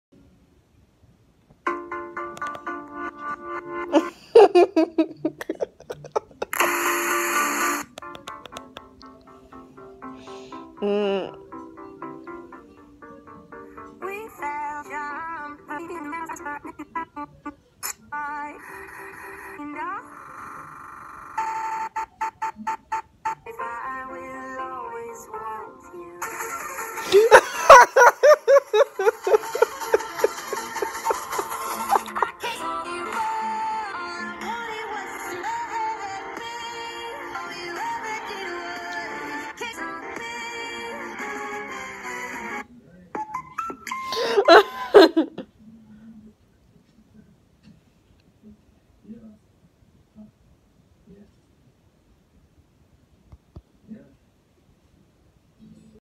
Reacționez La Un YouTube Poop Amuzant Cu Miley Cyrus!😂🤣
Acest YouTube Poop Este În Limba Engleza Americană